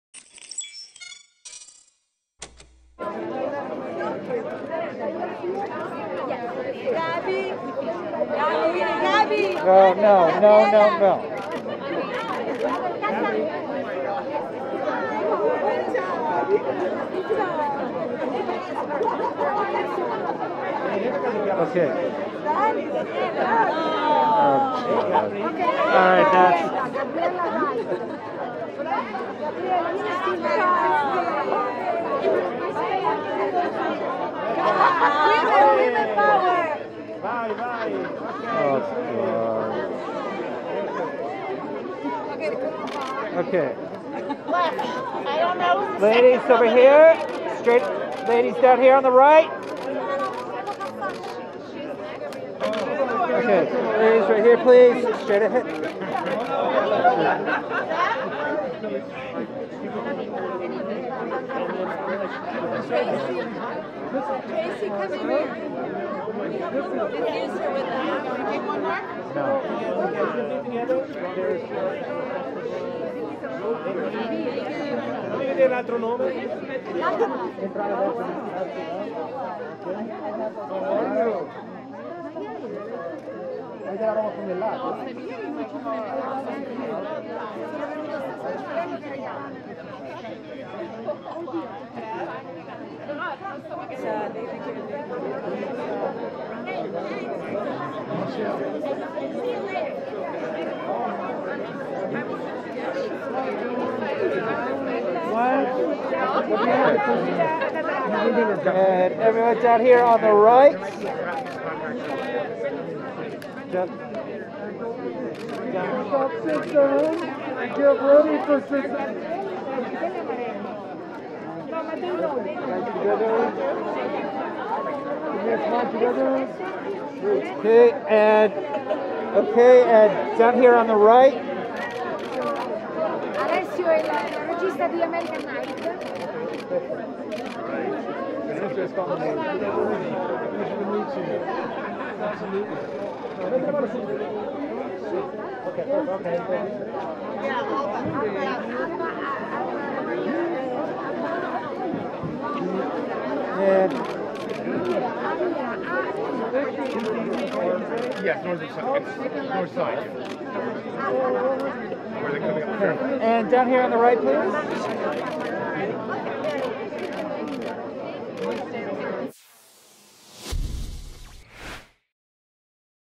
Tiziana Rocca's “Filming Italy Los Angeles 2022” Closing Night Red Carpet
Broll footage: Tiziana Rocca, Gisella Marengo, Marta Milans, Cecilia Peck, Inbal B Lessner, Tracie Laymon, Joe Cortese, Gabriella Wright, Gabriele Muccino, Will Brandt, Alessio della Valle on the red carpet at the Italian Cultural Institute's 7th Edition of ‘Filming Italy - Los Angeles’ closing night held at Harmony Gold Theater in Los Angeles, California USA on March 3rd, 2022

00:00 Intro
00:05 Gisella Marengo, Marta Milans
00:53 Cecilia Peck, Inbal B Lessner
01:09 Tracie Laymon
01:17 Tiziana Rocca
01:34 Gabriella Wright
01:49 Gabriele Muccino
01:59 Will Brandt
02:20 Alba Amira, Alessio della Valle
02:37 Joe Cortese